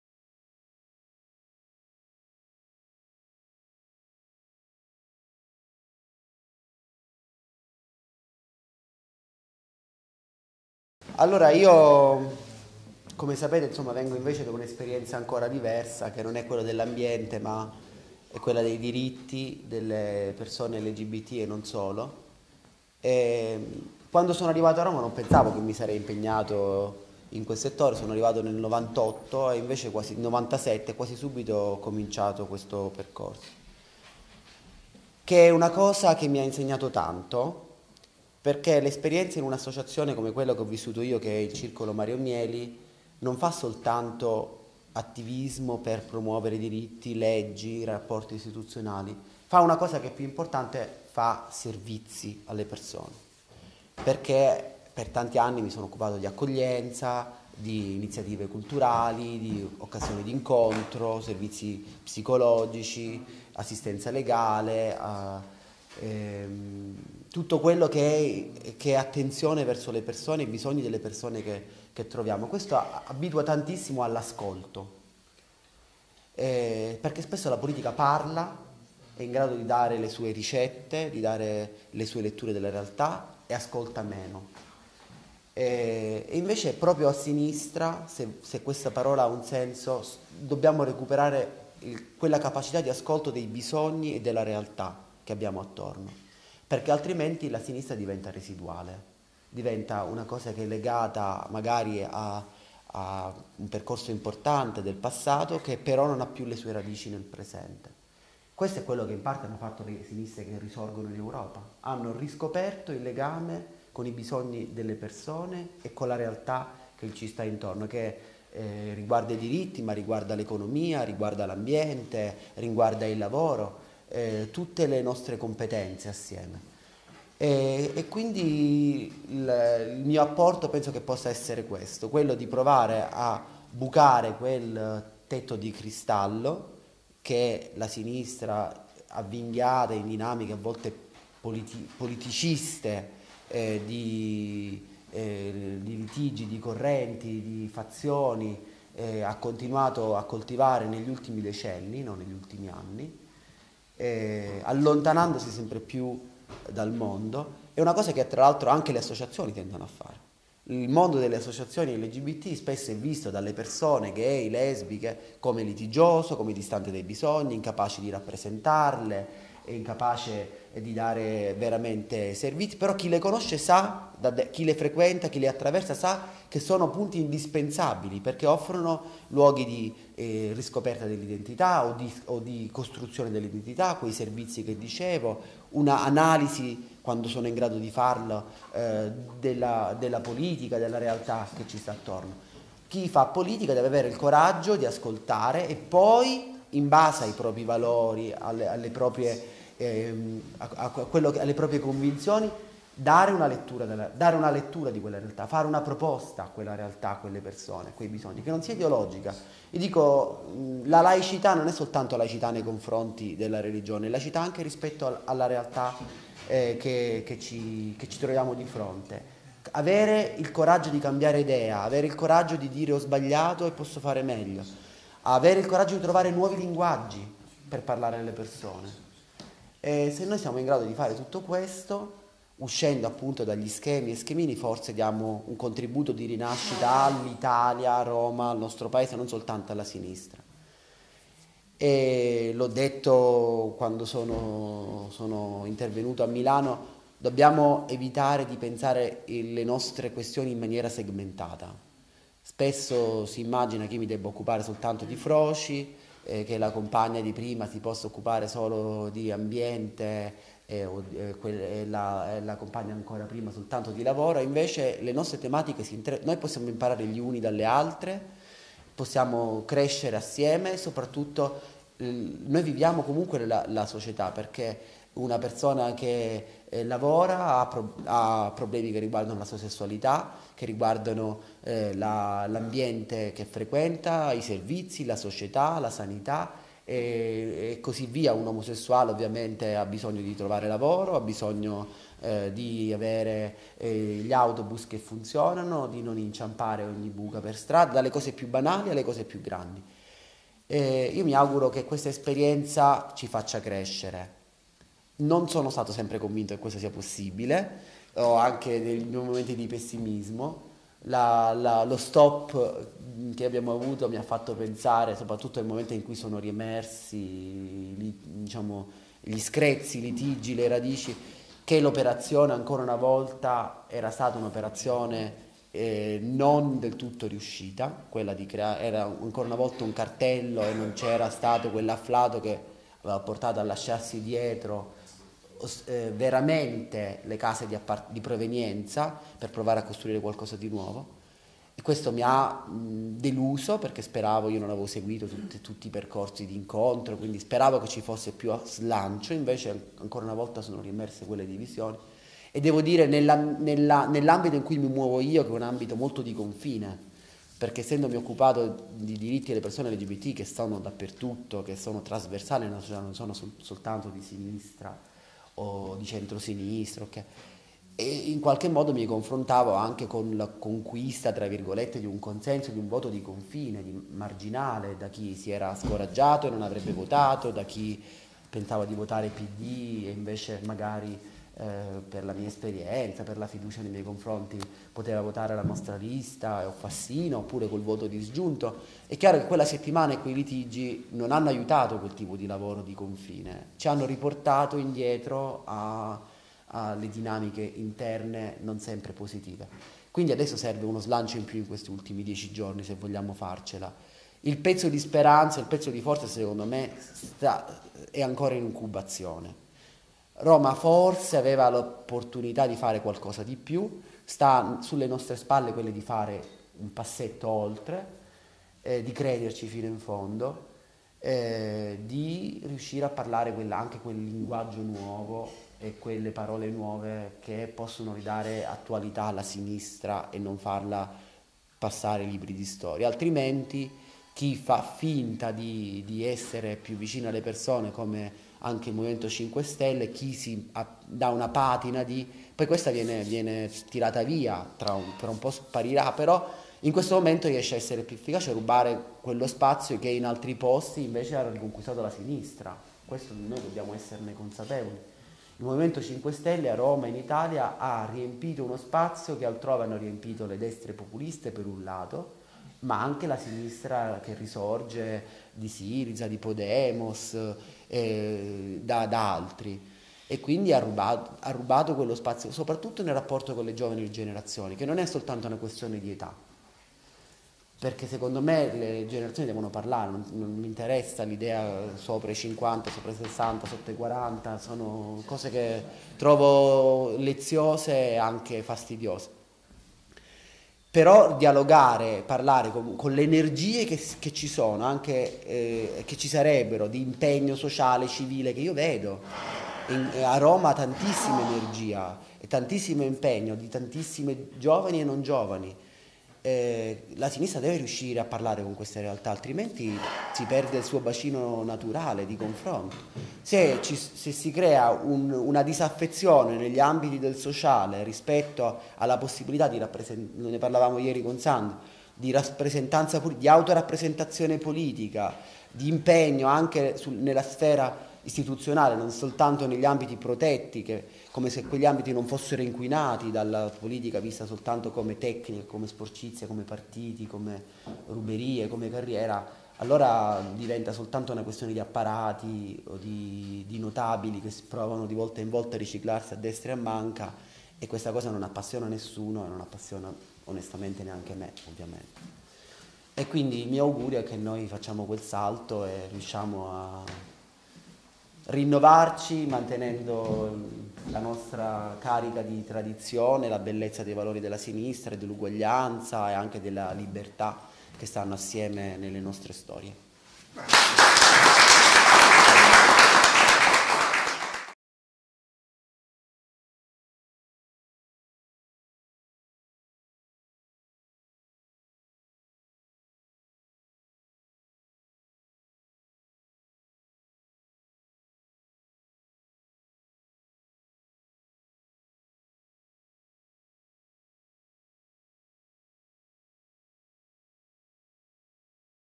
Andrea Maccarrone candidato Lista Sinistra x Roma - Fassina Sindaco
Presentazioni candidati elezioni Consiglio Comunale di Roma 5 giugno 2016.
Andrea Maccarrone per la lista Sinistra per Roma - Fassina Sindaco